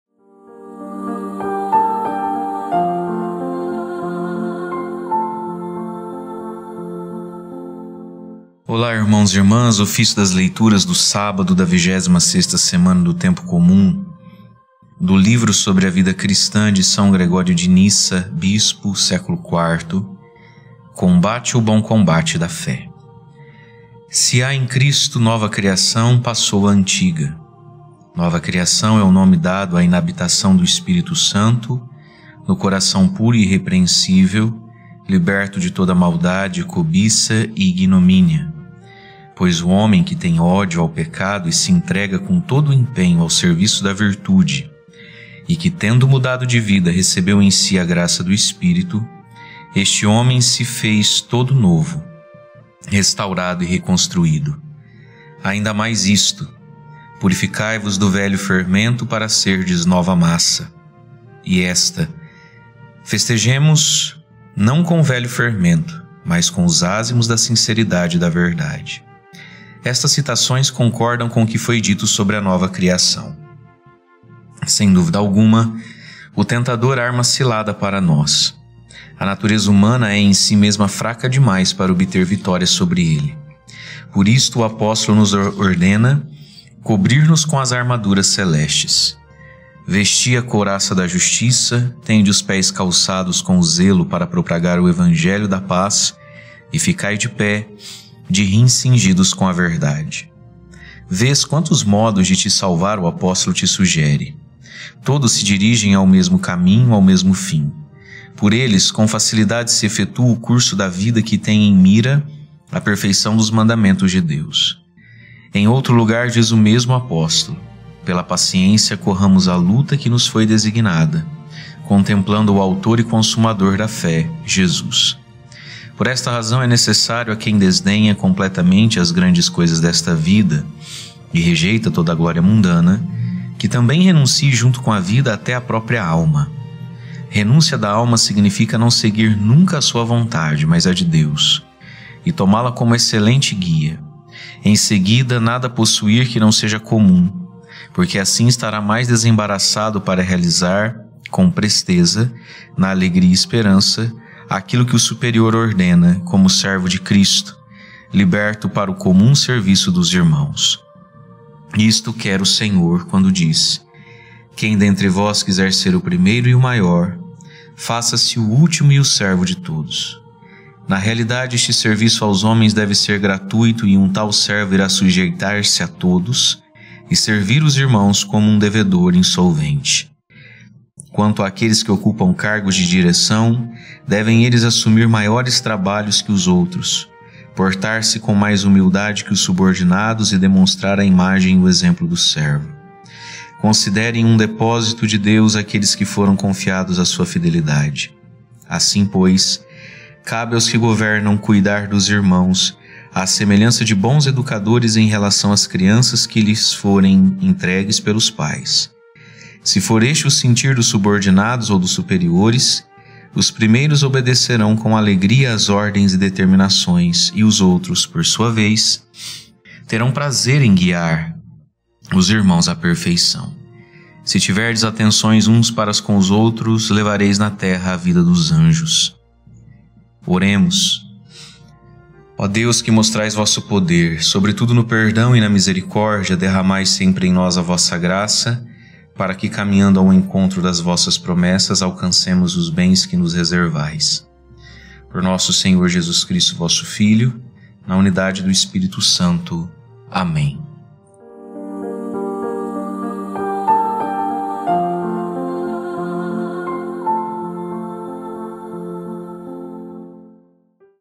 Ofício das Leituras | Sábado da 26ª Semana do Tempo Comum | Padre Mario Sartori
Do Livro sobre a vida cristã, de São Gregório de Nissa, bispo (Séc. IV)

Combate o bom combate da fé

Se há em Cristo nova criação, passou a antiga (2Cor 5,17). Nova criação é o nome dado à inabitação do Espírito Santo no coração puro e irrepreensível, liberto de toda maldade, cobiça e ignomínia. Pois o homem que tem ódio ao pecado e se entrega com todo o empenho ao serviço da virtude e que, tendo mudado de vida, recebeu em si a graça do Espírito, este homem se fez todo novo, restaurado e reconstruído. Ainda mais isto: Purificai-vos do velho fermento para serdes nova massa (1Cor 5,7). E esta: Festejemos, não com o velho fermento, mas com os ázimos da sinceridade e da verdade (1Cor 5,8). Estas citações concordam com o que foi dito sobre a nova criação.

Sem dúvida alguma o tentador arma ciladas para nós. A natureza humana é em si mesma fraca demais para obter vitória sobre ele. Por isto o Apóstolo nos ordena cobrir-nos com as armaduras celestes: Vesti a couraça da justiça, tende os pés calçados com o zelo para propagar o Evangelho da paz e ficai de pé, de rins cingidos com a verdade (cf. Ef 6,14). Vês quantos modos de te salvar o Apóstolo te sugere. Todos se dirigem ao mesmo caminho, ao mesmo fim. Por eles com facilidade se efetua o curso da vida que tem em mira a perfeição dos mandamentos de Deus. Em outro lugar diz o mesmo Apóstolo: Pela paciência corramos à luta que nos foi designada, contemplando o autor e consumador da fé, Jesus (Hb 12,1-2).

Por esta razão é necessário a quem desdenha completamente as grandes coisas desta vida e rejeita toda glória mundana, que também renuncie junto com a vida até à própria alma. Renúncia da alma significa não seguir nunca a sua vontade mas a de Deus, e tomá-la como excelente guia. Em seguida, nada possuir que não seja comum. Porque assim estará mais desembaraçado para realizar com presteza, na alegria e esperança, aquilo que o superior ordena, como servo de Cristo, liberto para o comum serviço dos irmãos. Isto quer o Senhor, quando diz: Quem dentre vós quiser ser o primeiro e o maior, faça-se o último e o servo de todos (cf. Mc 9,35).

Na realidade, este serviço aos homens deve ser gratuito, e um tal servo irá sujeitar-se a todos e servir os irmãos como um devedor insolvente. Quanto àqueles que ocupam cargos de direção, devem eles assumir maiores trabalhos que os outros, portar-se com mais humildade que os subordinados e demonstrar a imagem e o exemplo do Servo. Considerem um depósito de Deus aqueles que foram confiados à sua fidelidade.

Assim, pois, cabe aos que governam cuidar dos irmãos, à semelhança de bons educadores em relação às crianças que lhes foram entregues pelos pais. Se for este o sentir dos subordinados ou dos superiores, os primeiros obedecerão com alegria às ordens e determinações, e os outros, por sua vez, terão prazer em guiar os irmãos à perfeição. Se tiverdes atenções uns para com os outros, levareis na terra a vida dos anjos.